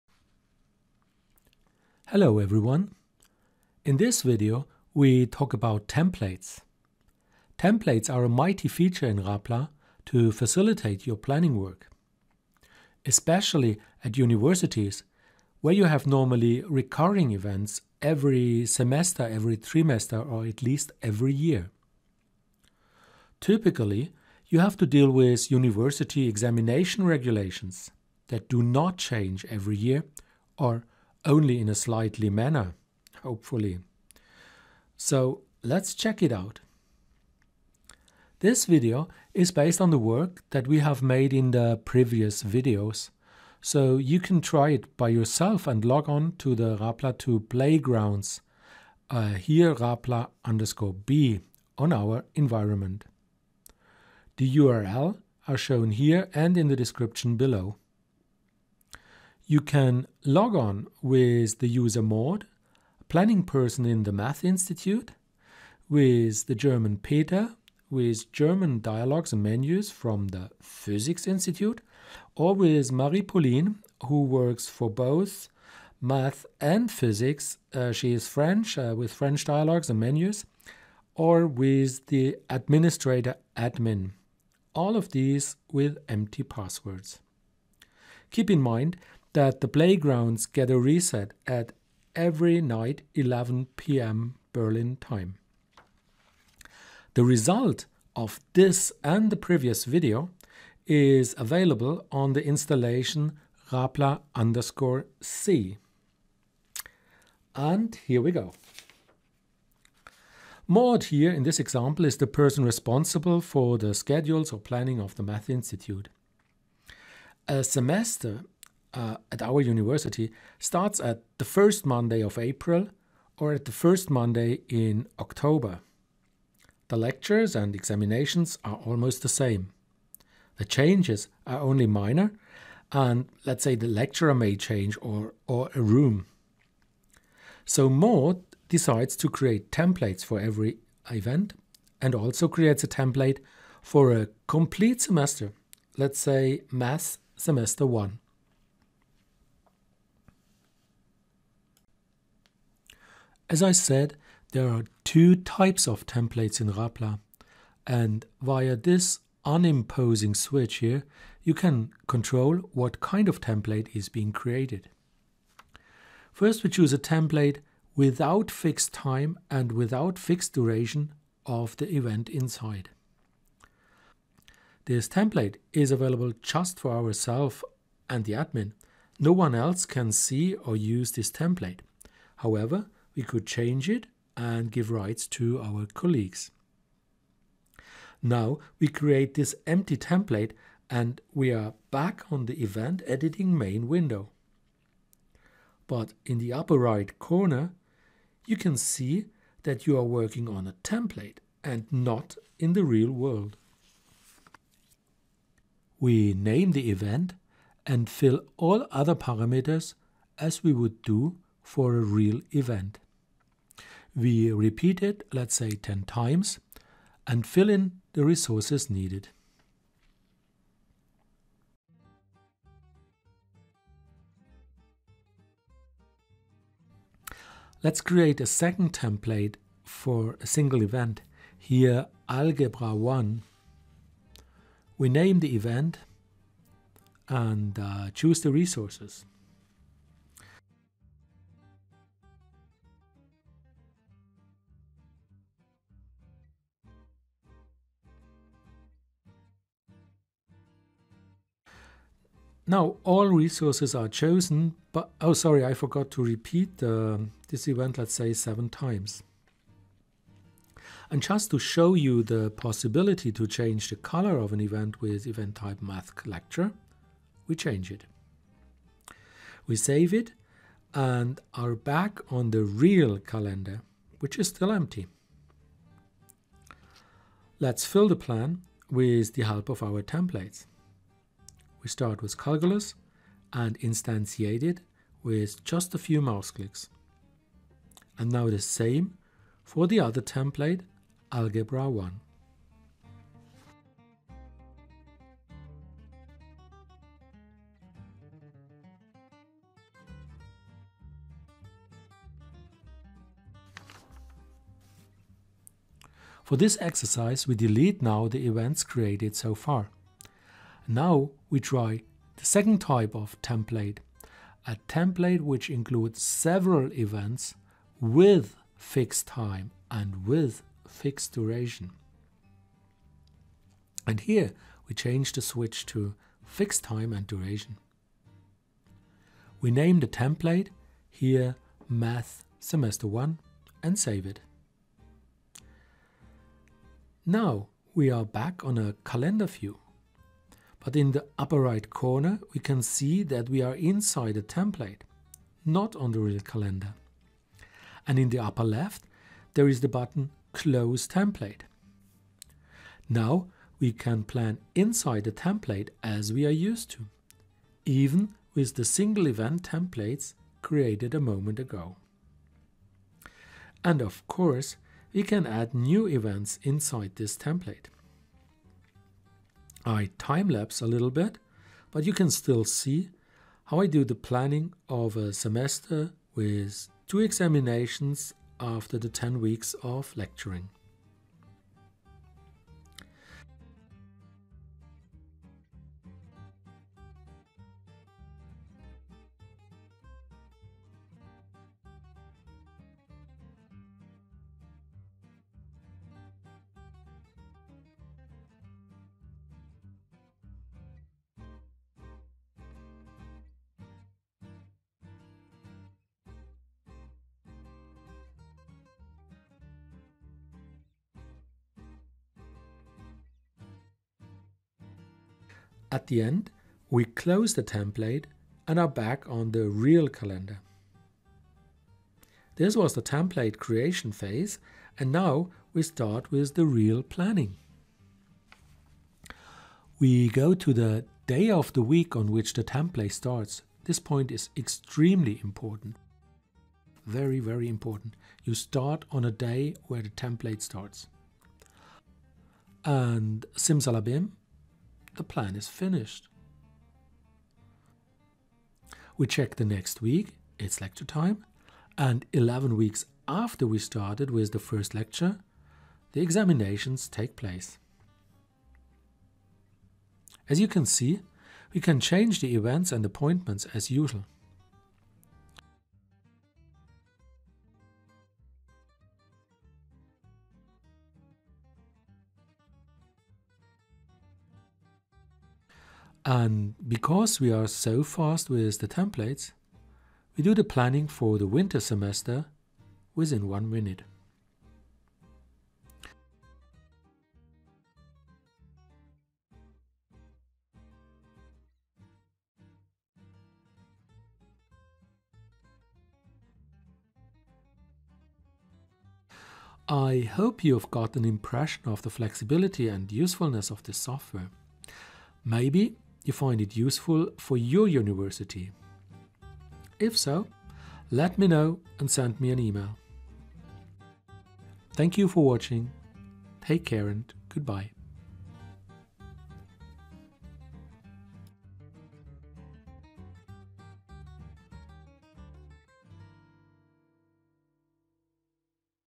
Rapla2 - Introduction for Users: Planning with Templates
Rapla2 is a flexible multi-user resource and event planning system.

It features multiple calendars views, conflict-management, fully configurable resource and event types and many import/export functions.

It is used at universities, in hospitals for operating theatre planning, in international training institutes and many more.

Version 2 of Rapla now works with new Java11 Webstart provided on openwebstart.com and supports many European languages.

In this video, we explain the use of templates. A very useful feature, particulary when there are recurring events.

(this is the situation after this video)

Log in with "admin" and empty password. Login with "maude", "peter" and "marie-pauline" also without password.
Feel free and play around. There will be a reset every night at 11 pm (Berlin time).